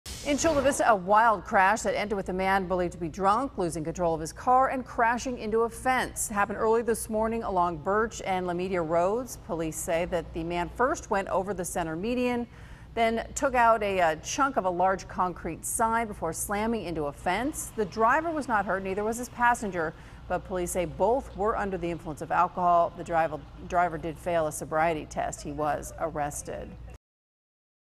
DUI Suspect Arrested After Violent Crash
A driver under the influence of alcohol crashed into a sign, according to the Chula Vista Police Department.